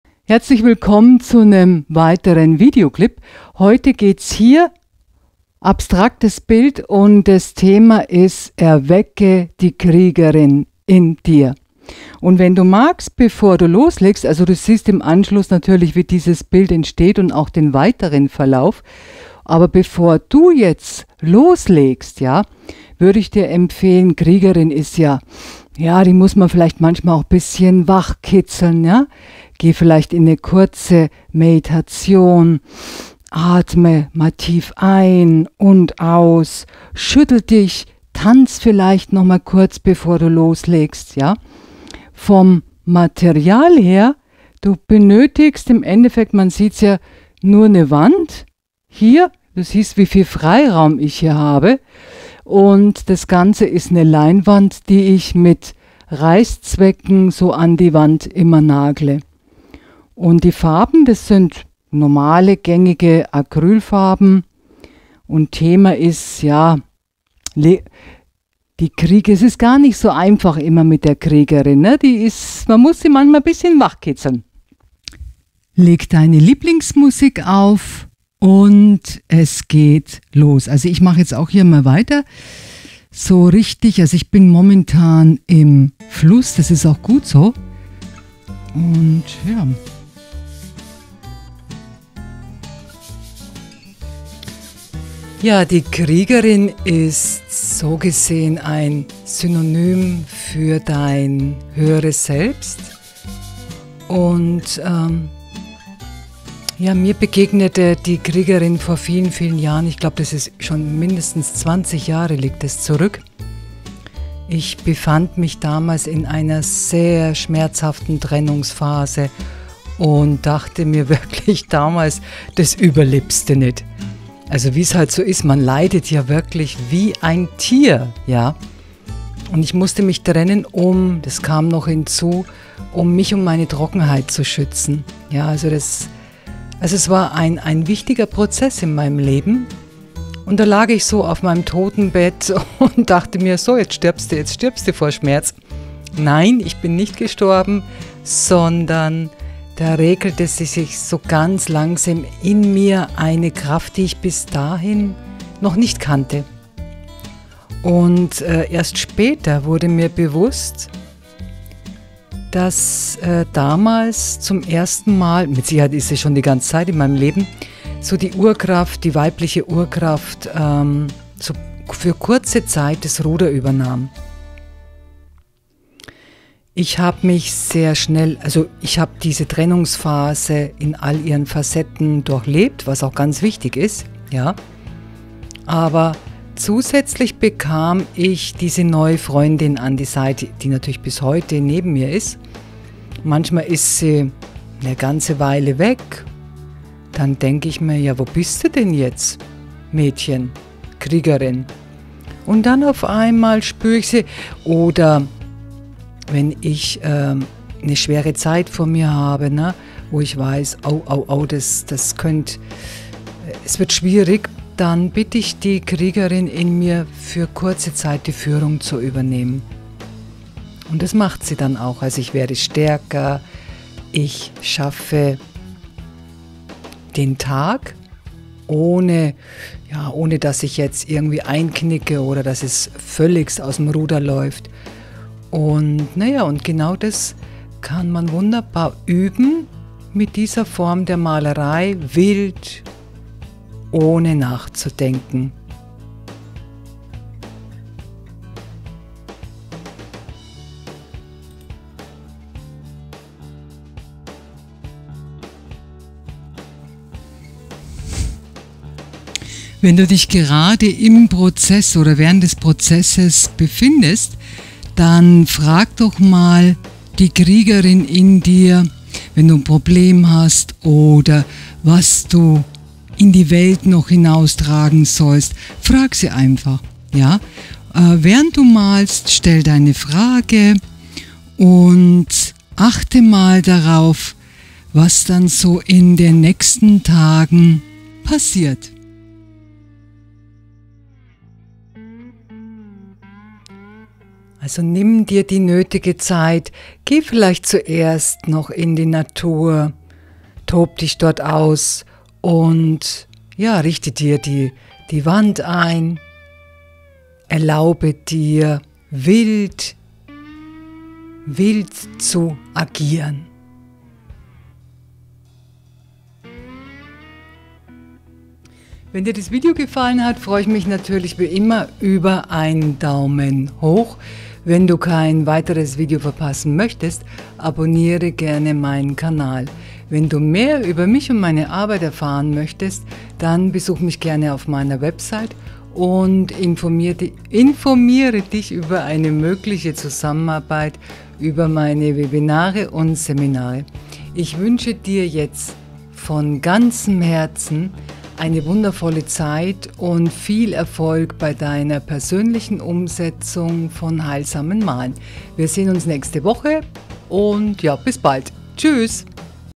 I Erwecke die Kriegerin in Dir I Step by step I Intuitiv malen
Erlaube dir heute wild und frei zu leben.
In diesem Video erfährst du, wie du einen liebevollen Kontakt zu deiner inneren Kriegerin herstellst. Auch für Anfänger.

➡️ Meine Angebote